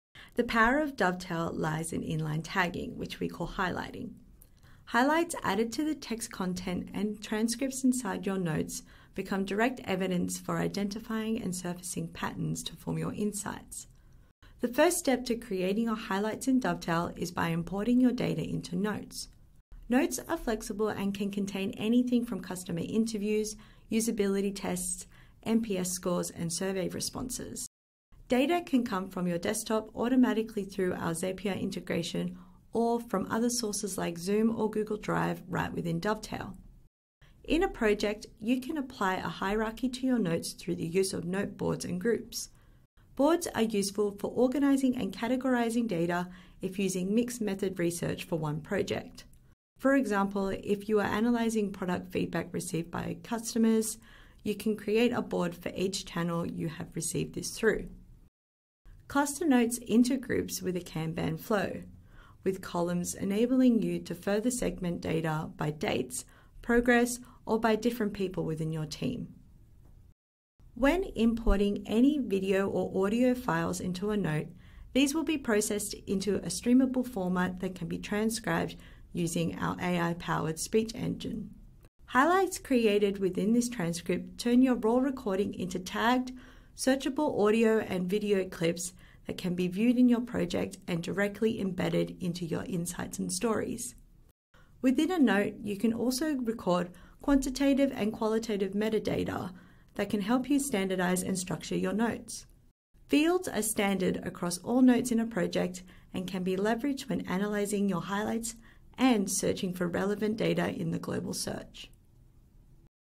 How-to with Emily: Notes
In this quick demo, learn how to store your research data in Dovetail's 'notes' from Emily Brogan, our Inbound Customer Specialist.
Hungry for more? Check out a deeper look into notes